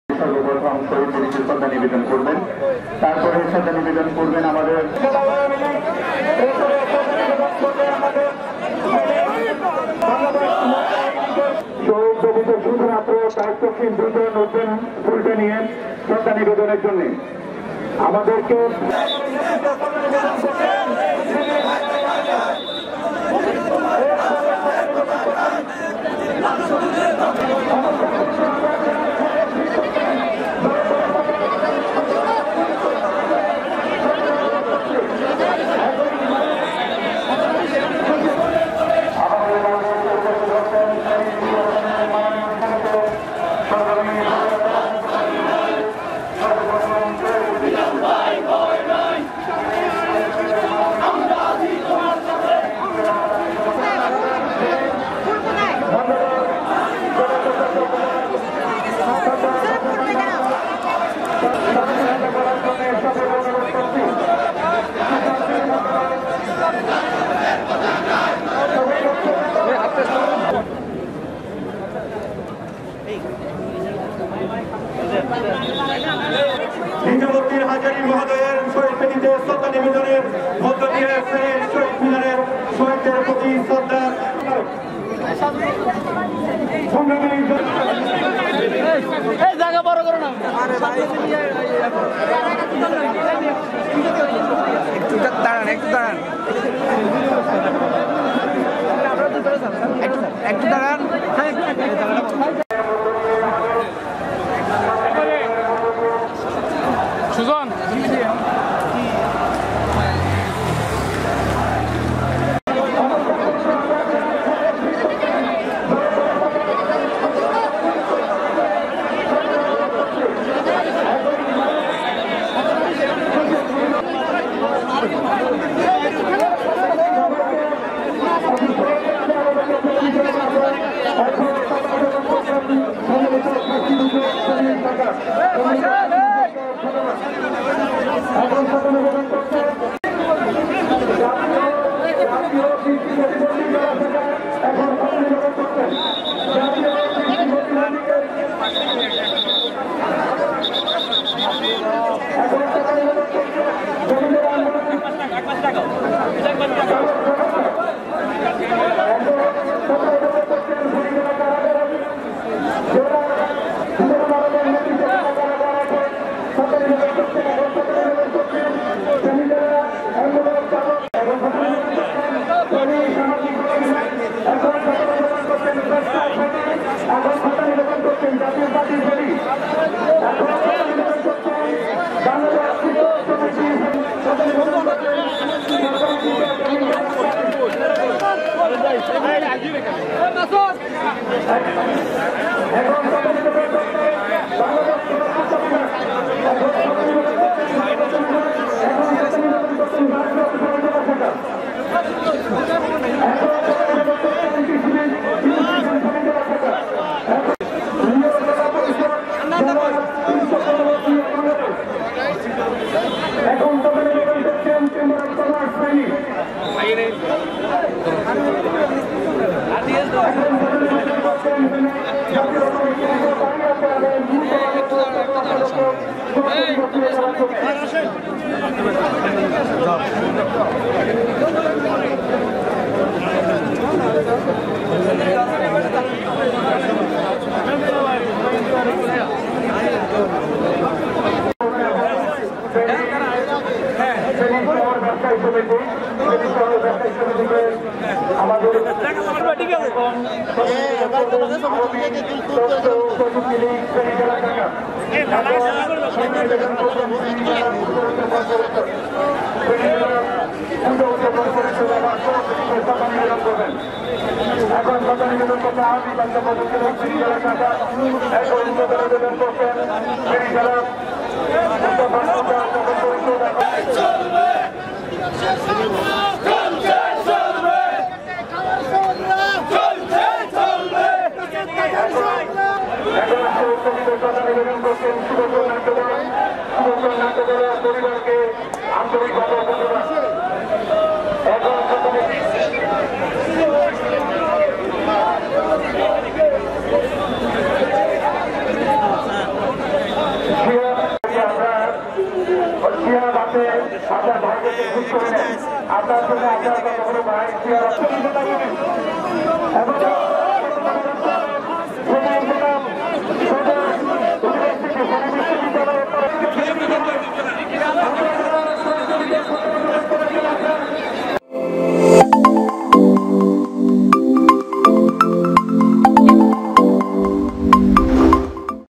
21 February Feni Shohid Miner
21 February Feni Shohid Miner
Festive throngs of the Feni Shaheed Minar on International Mother Language Day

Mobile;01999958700